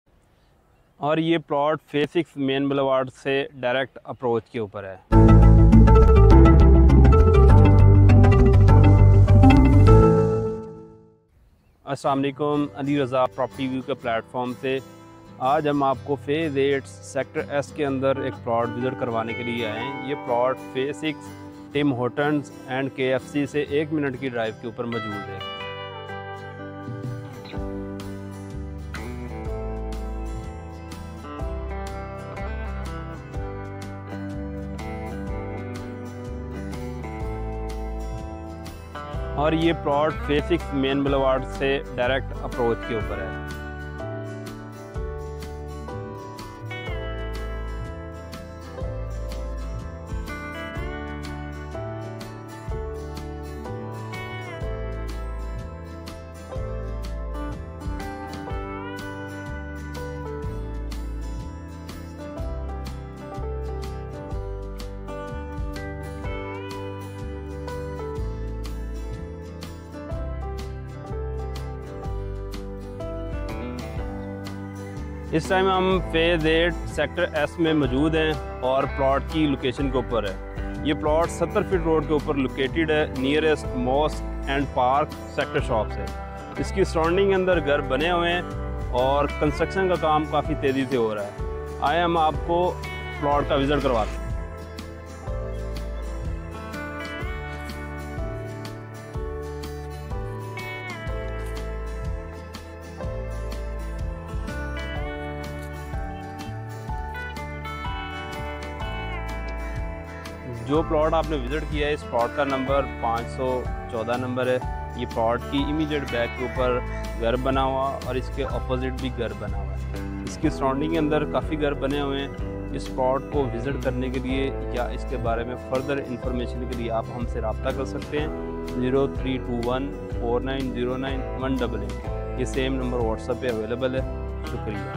DHA PHASE 8 BLOCK S PLOT ON 70' FEET ROAD ALI RAZA 03214909188
Hot Location Plot For Sale in DHA Phase 8 Block-S
Property Features
* Near to KFC and Tim Horton's Phase-6 Branch
* Near to Main Mosque and Park
* Direct Approach from Main boulevard
* Property is located on 70' road
*Surrounded with constructed Houses
For more details and visit call @ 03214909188 (ALI RAZA)